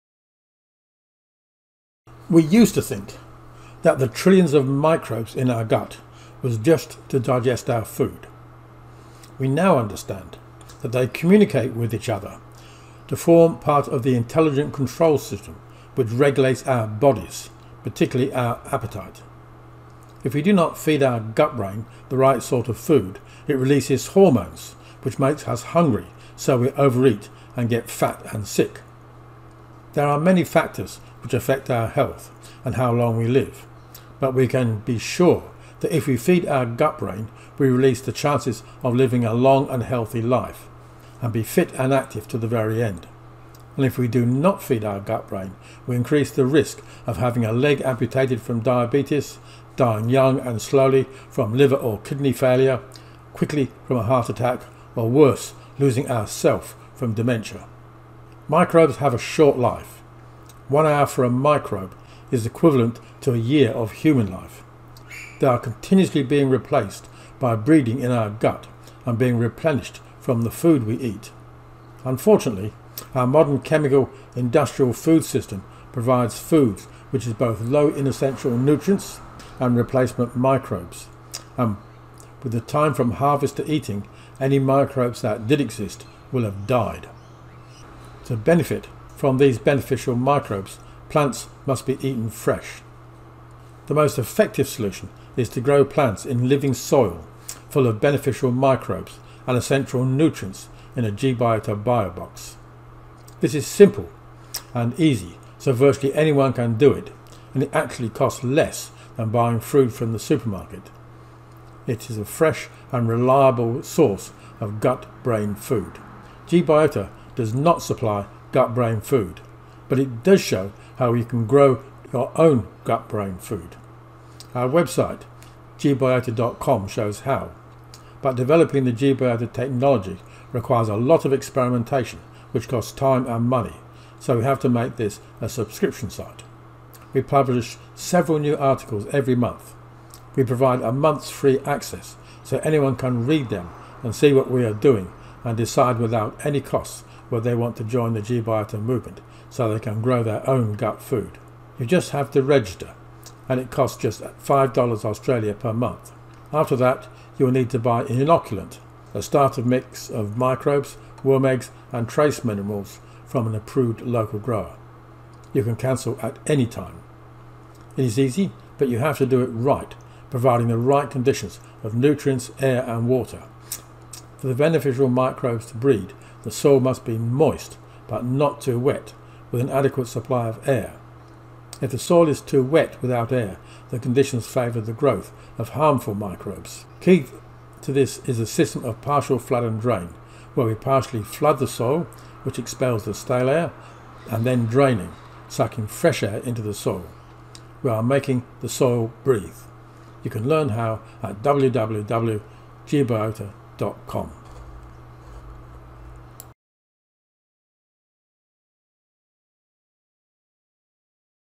Grow gut microbes
How to improve health by growing gut microbes yourself at home - it is easy when you know how